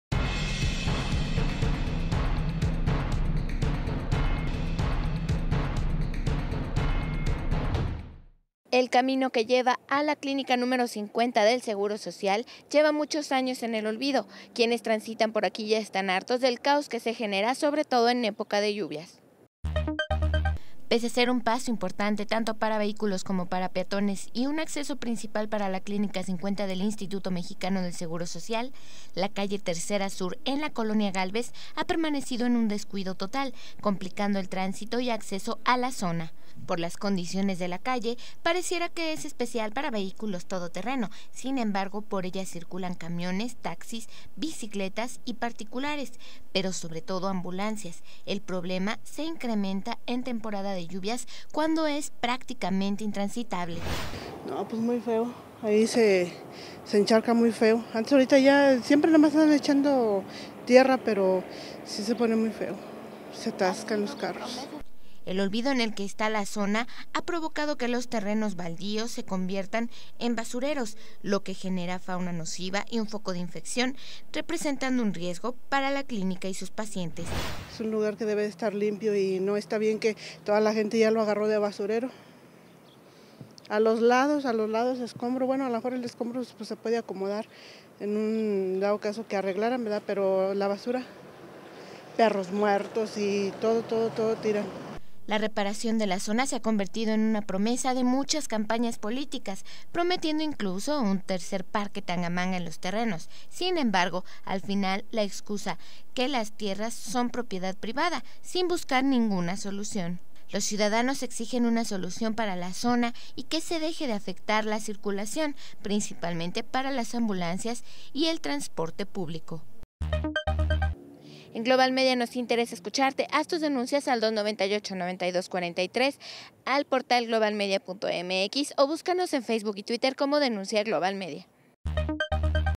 Un basurero accesos a Clínica 50 del IMSS
La calle Tercera Sur sufre de inundaciones y una gran cantidad de residuos que provocan fauna nociva